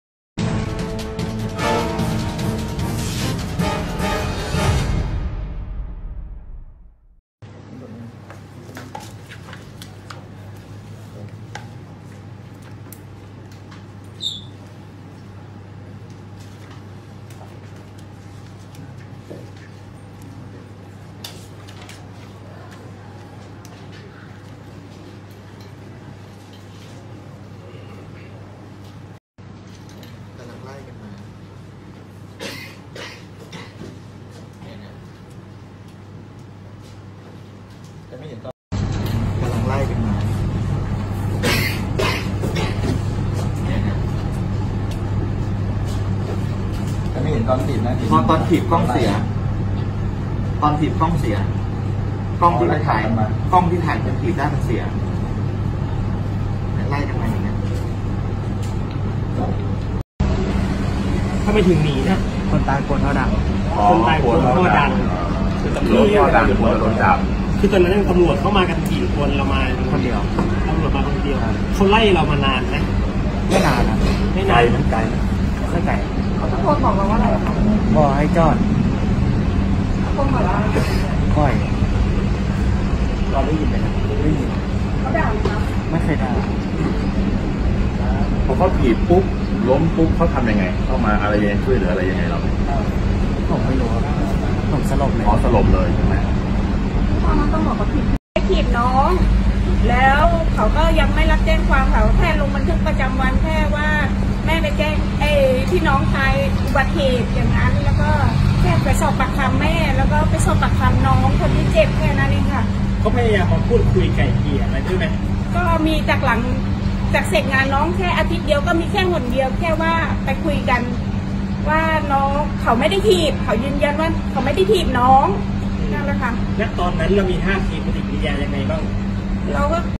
ร้องกองปราบปราม ลูกถูกตำรวจถีบ จยย.ล้มจนเสียชีวิต
29/01/2019 ร้องกองปราบปราม ลูกถูกตำรวจถีบ จยย.ล้มจนเสียชีวิต
MGR online - พ่อ แม่ โจ๋ วัย 16 ร้องกองปราบ ลูกถูกตำรวจ สภ.บ้านโป่ง ทำเกินกว่าเหตุ ถีบรถ จยย.ล้มจนเสียชีวิต แค่รถท่อดัง